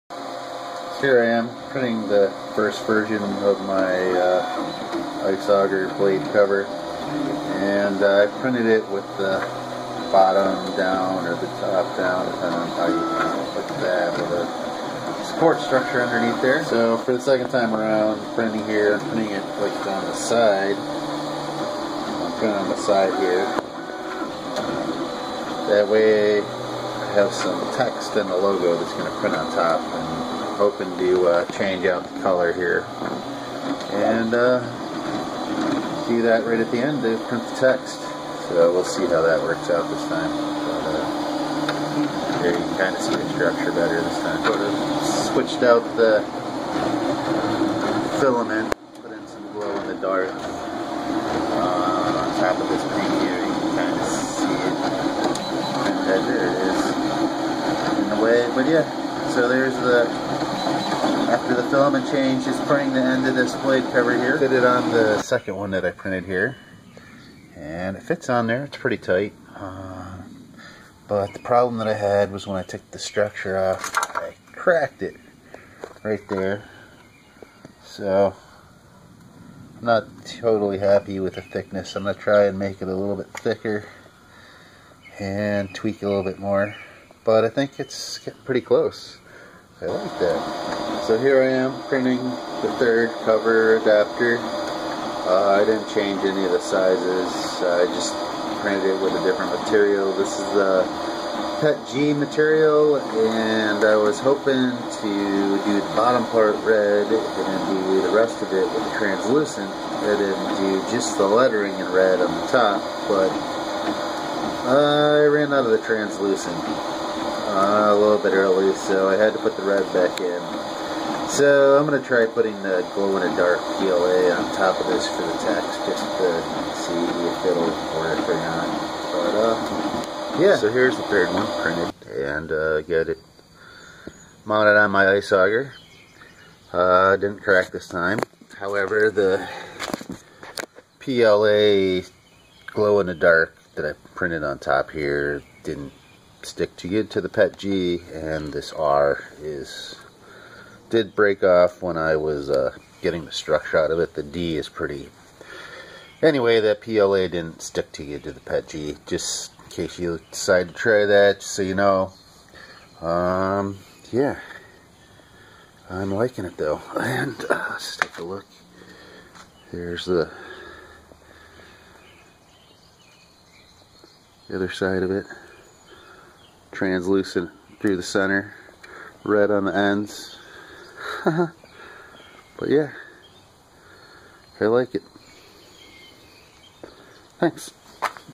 3D Printed Ice Auger Blade Cover
First 2 versions printed with PLA.  The final 3rd version printed with PETG.

I believe the auger in this video is an Eskimo brand but his guard should fit most 6" straight blade hand ice augers including Eskimo, Mora, Strikemaster, Rapala, ThunderBay and HT.